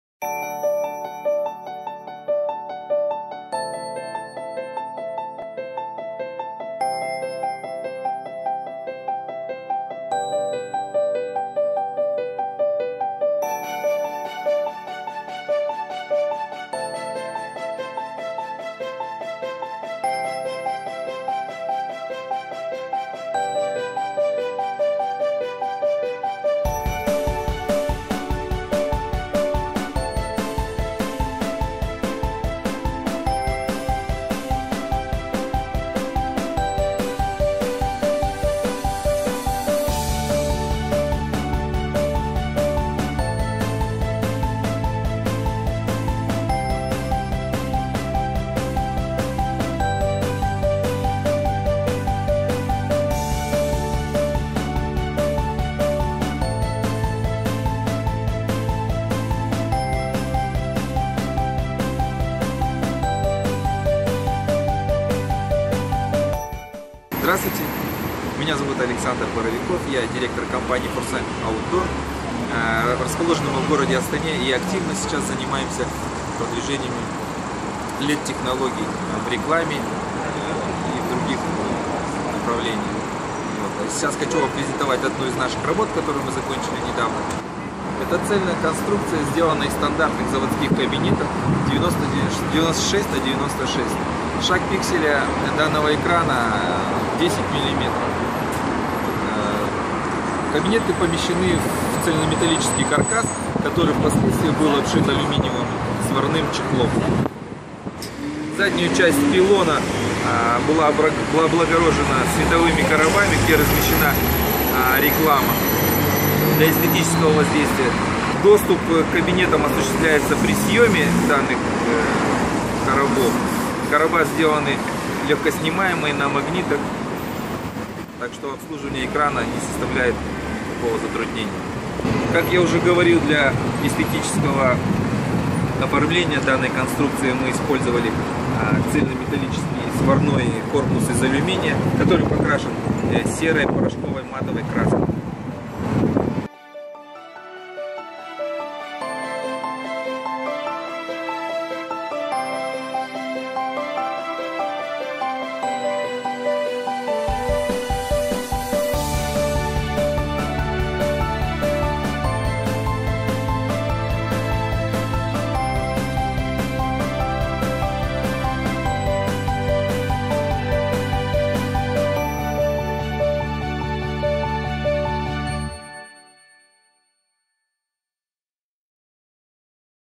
Kazakhstan. Astana. Ost West
Светодиодный экран в виде отдельно стоящей конструкции.
Размер экрана: высота 288 см, ширина 192 см
Резолюция экрана: 288*192
В данной конструкции использованны: 6 стандартных кабинетов 96см*96см Шаг пикслея: 10 мм.
Авиакассы по адресу: пр. Республики уг. Иманова